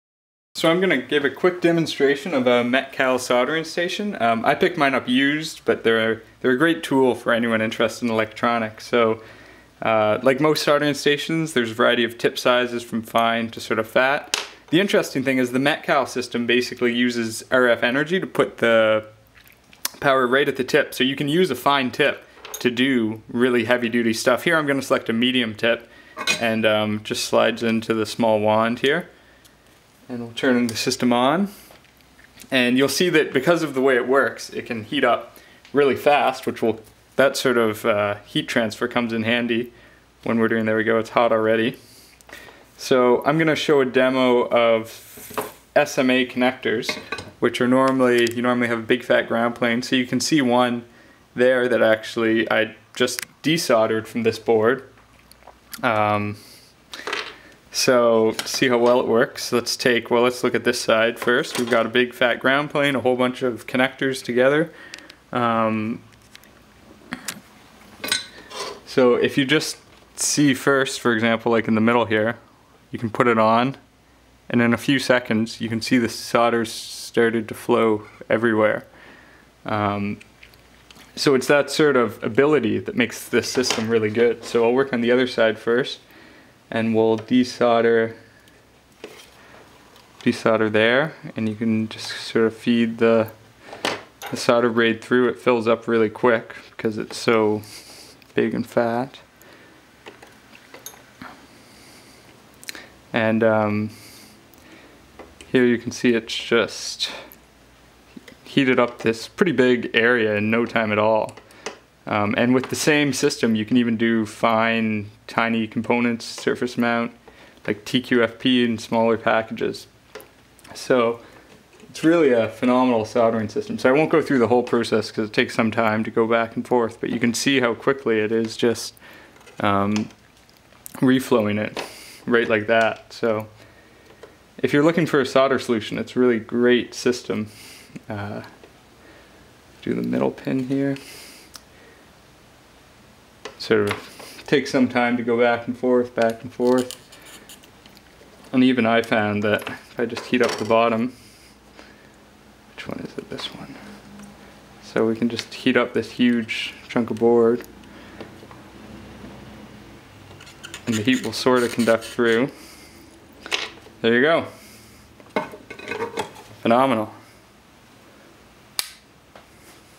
METCAL MX500 Example #1: Introduction and Desoldering SMA With Braid
for more, and check out part 2 for a super-fast example of desoldering an SMA connector Uses a METCAL MX500-P I bought used, METCAL probably has the best hand-soldering station in the world.